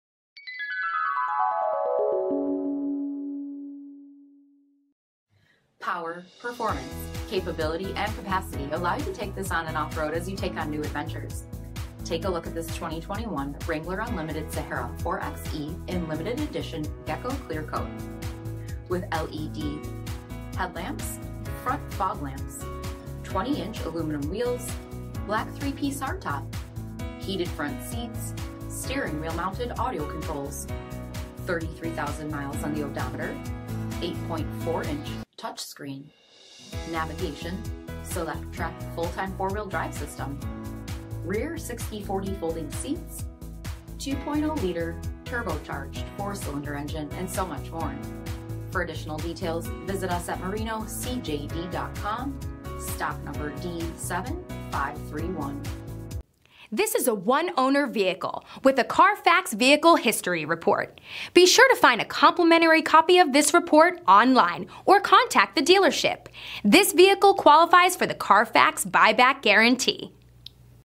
2021 Jeep Wrangler D7531
2021 Jeep Wrangler Unlimited Sahara 4xe

5133 W Irving Park Rd

WRANGLER UNLIMITED SAHARA 4XE!!  7 YEAR - 100,000 MILE WARRANTY!! 2.0 LITER TURBOCHARGED 4 CYLINDER ENGINE!!  STILL UNDER FACTORY POWERTRAIN WARRANTY!!  ONE OWNER!!!  ACCIDENT FREE CARFAX REPORT!!  COLD WEATHER GROUP!!  HEATED FRONT SEATS!!  REMOTE START SYSTEM!!  BLACK FREEDOM TOP 3 PIECE HARDTOP!!  SIDE STEPS!!  8.4 INCH TOUCH SCREEN DISPLAY!!  NAVIGATION!!!  REAR BACKUP CAMERA!!  APPLE CARPLAY!!  GOOGLE ANDROID AUTO!!  LED LIGHTING GROUP!!  STEERING WHEEL MOUNTED AUDIO CONTROLS!!  POWER LOCKS!!!  POWER WINDOWS!!